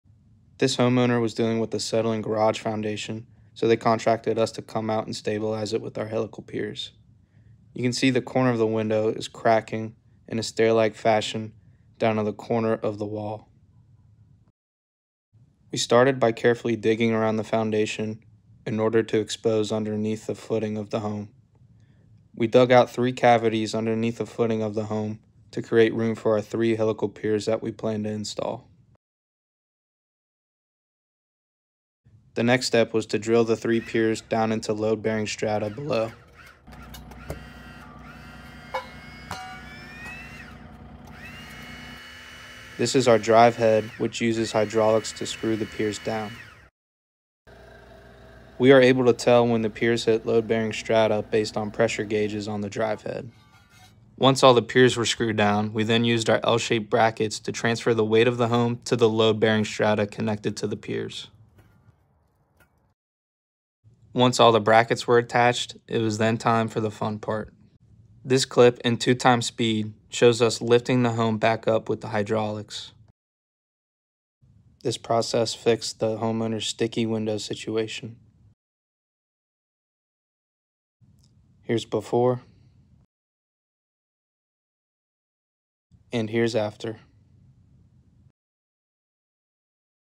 Helical Pier Underpinning: Foundation Repair Project Explained in 90 Seconds
This exterior garage wall had a crack spanning from the bottom right corner of the window down to the corner of the brick wall. This crack formed in a stair-step fashion, which is an immediate tell of foundation settlement. As a result, this window was no longer functional, as it couldn't open or close. We fixed the problem by underpinning their foundation with 3 helical piers, right underneath the window. After stabilizing the home, we then lifted the foundation back up to fix the window and to close the cracks. The homeowner was able to open and close the window afterwards, and was super impressed with how much of a difference our piers made.